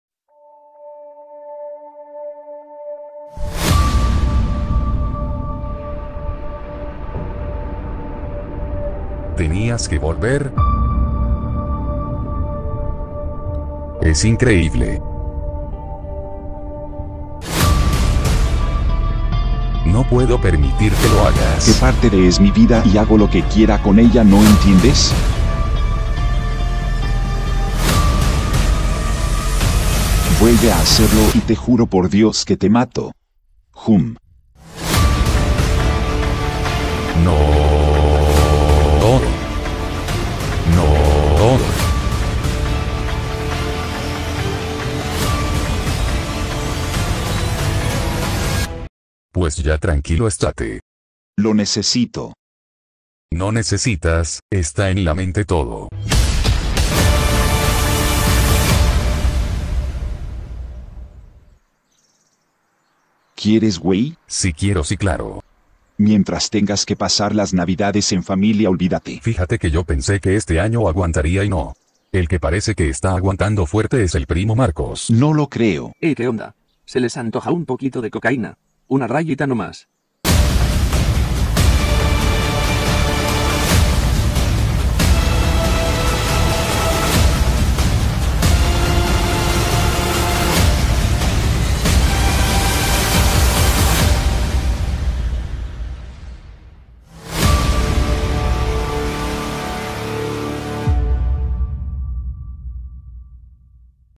Trailer "Navidad" (AUDIO CORREGIDO)
Hace un par de meses subimos este video pero muchos os quejasteis porque el audio estaba en español latino.
Gracias a un sofisticado programa de edición de sonido controlado por una inteligencia artificial hemos podido corregirlo y el resultado es increíble.